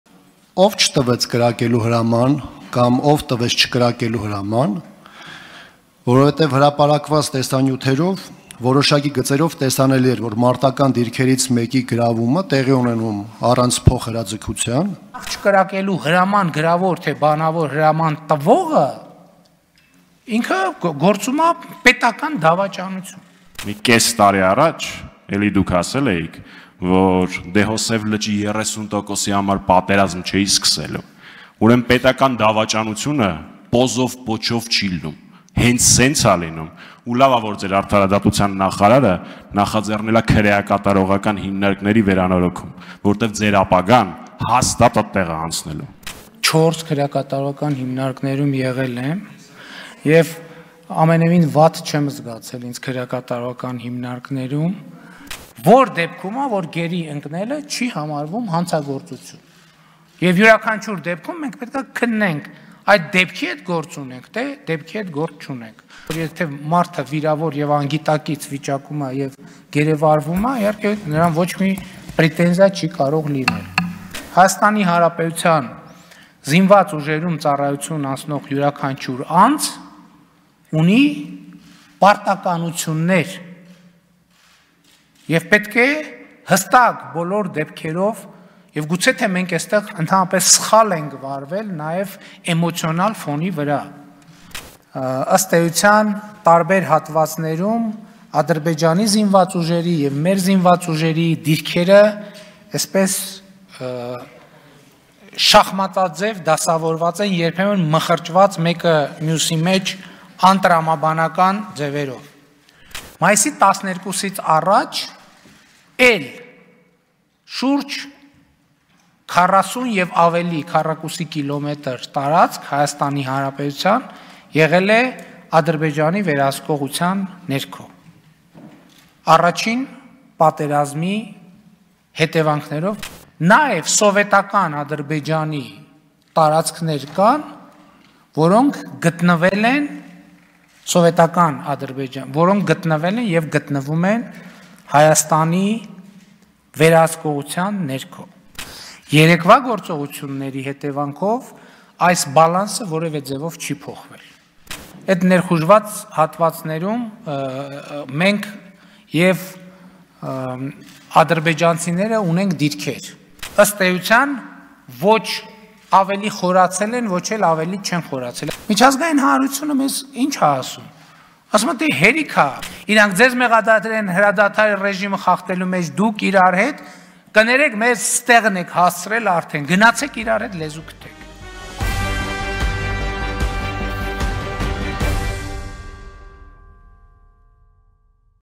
«Ադրբեջանցիները ՀՀ տարածքում ոչ ավելի խորացել են, ոչ էլ ավելի չեն խորացել». Փաշինյանը՝ ԱԺ-ում
Ըստ Հայաստանի վարչապետի՝ երեկվա մարտական գործողություններից հետո սահմանային իրավիճակում բալանսը չի խախտվել, թեև դիրքային փոփոխություններ եղել են:

Երեկ ադրբեջանական կողմում հայտնված գերիների ճակատագրի վերաբերյալ տեղեկություններ հայտնելու փոխարեն էլ Փաշինյանը խորհրդարանի ամբիոնից կասկածի տակ է դրել նրանց՝ գերի ընկնելու հանգամանքները: